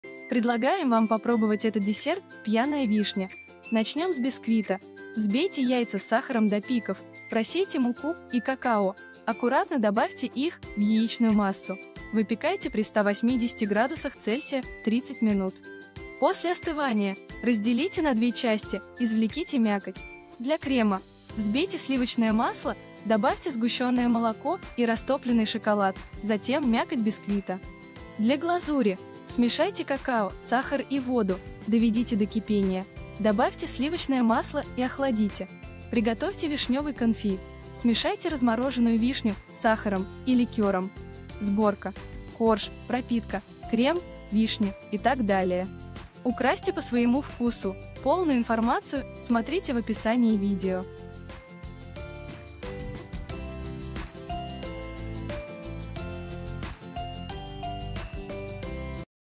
Идеально вкусный торт «Пьяная вишня»: пошаговый рецепт
Мы нашли это интересное видео, которое непременно стоит попробовать! Сегодня спешим поделиться невероятно вкусным рецептом торта **«Пьяная вишня»**. Понравится всем любителям сочных и насыщенных тортов.

1. Взбейте яйца с сахаром до устойчивых пиков (примерно 5-7 минут).
2. Просейте муку и какао.
3. В три приёма введите муку и какао в яичную массу, аккуратно перемешивая лопаткой.
4. Выпекайте в разогретой до 180°C духовке около 30 минут (готовность проверяйте зубочисткой).
5. Выньте бисквит из формы спустя 10 минут после выпекания, полностью остудите, заверните в пищевую плёнку и уберите в холодильник на несколько часов (лучше на ночь).
6. Разделите на 2 части, извлеките мякоть для крема и посыпки по бокам.

### Крем:
- Сливочное масло - 250 гр.
- Сгущенное молоко - 1 банка
- Растопленный шоколад - 50 гр. или глазурь

1. Все ингредиенты должны быть комнатной температуры. Взбейте сливочное масло, постепенно добавляя сгущенное молоко до получения крема.
2. Введите растопленный шоколад или глазурь.
3. Добавьте мякоть бисквита.

1. Смешайте какао и сахар, добавьте воду и перемешайте. Варите на среднем огне до закипания, не кипятите.
2. Снимите с огня и через 3-5 минут добавьте масло, перемешайте.
3. Оставьте остывать.

### Вишневый конфи:
- Вишня (без косточек, замороженная)
- Сахар - 100 гр.
- Ром/ликер - 100 мл.

1. Добавьте к размороженной вишне сахар и ликёр, перемешайте до растворения сахара.
2. Пропитайте коржи выделившимся соком.

### Сборка:
1. Корж - пропитка вишневым соком - крем - вишня - корж - пропитка - крем (после этого уберите торт в холодильник минимум на 3 часа) - полейте глазурью.
2. Украсьте торт на свой вкус.

Нереально вкусный торт с необычным вкусом и оформлением! Простой и быстрый рецепт, как и все наши рецепты. Обязательно приготовьте и поделитесь своими впечатлениями! Приятного аппетита!

C4w7cfLtNQA